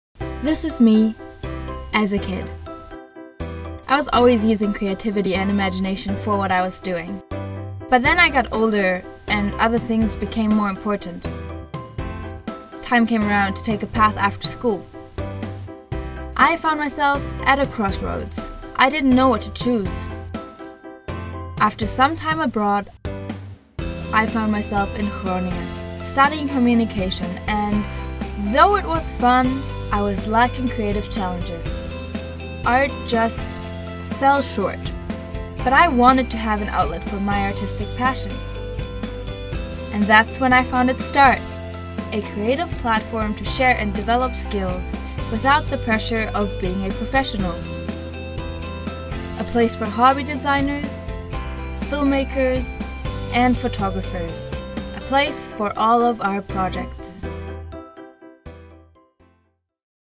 International Communication - pitch video block 1.3
Have a look at how creative students from the International Communication Hanze UAS programme can be: this was made by first year student Jelena Queissner during the first year.